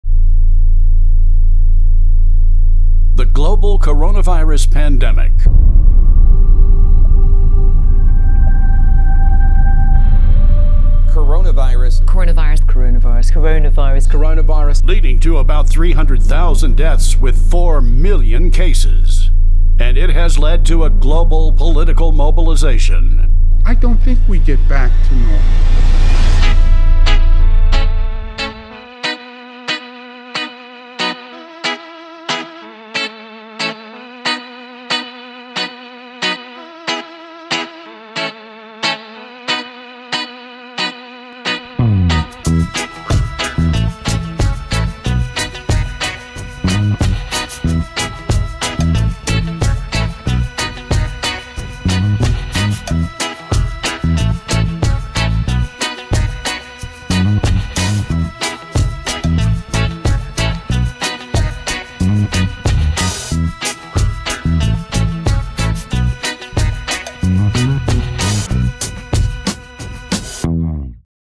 Global Teach In 2020
Promotional video for "Global Teach-In 2020: Democratize the Crisis," May 26, 2020.